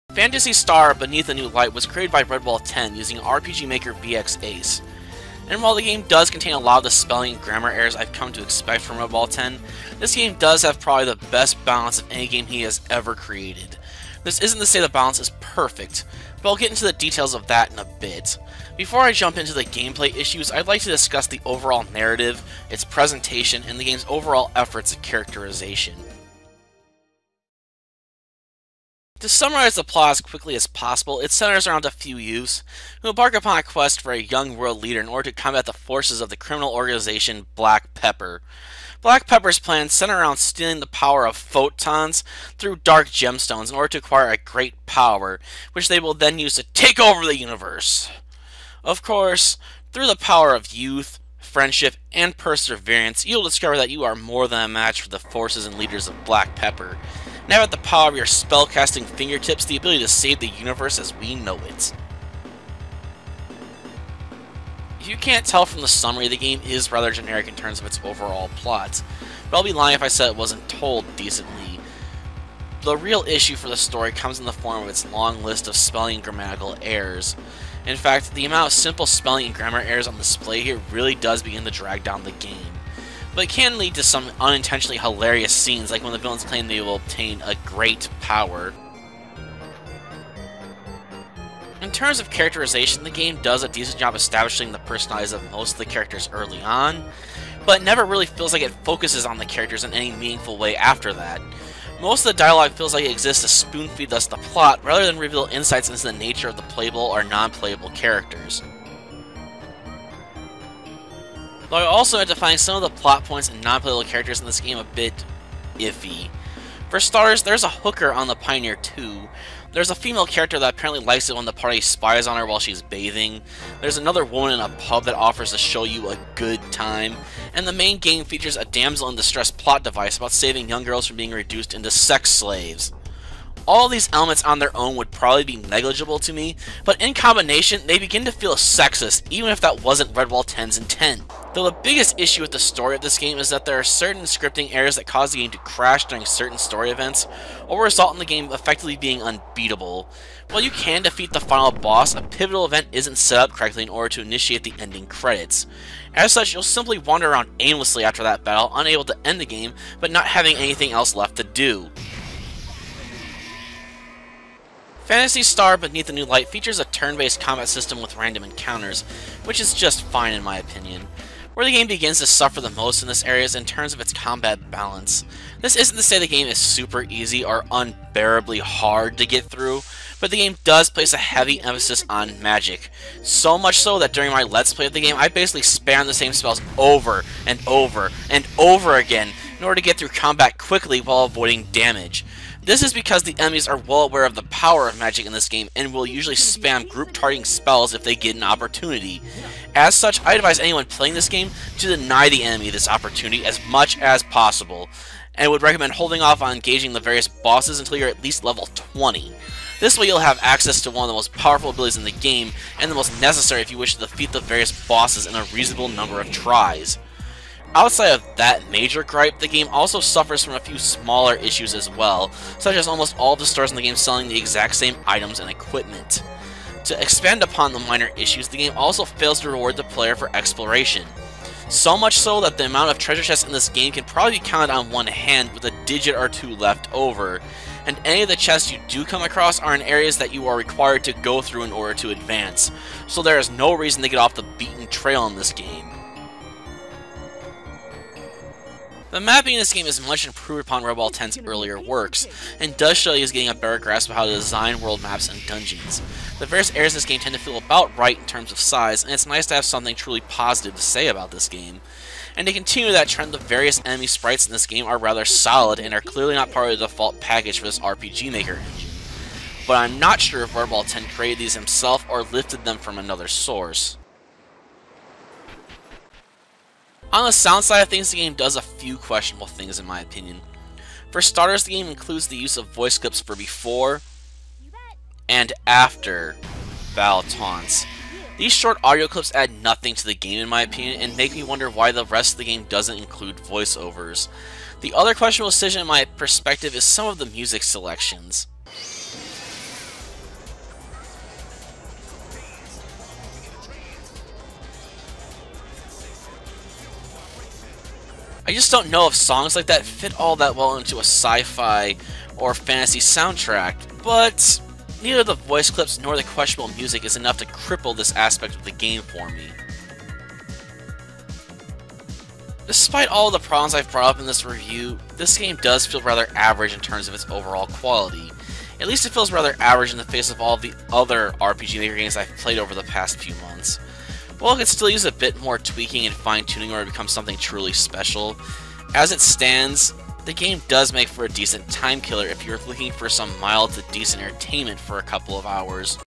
Sacred Reviews: Phantasy Star - Beneath a New Light
A review for Phantasy Star - Beneath a New Light by redwall10.  If this review interests you in playing the game it can be downloaded at the following web address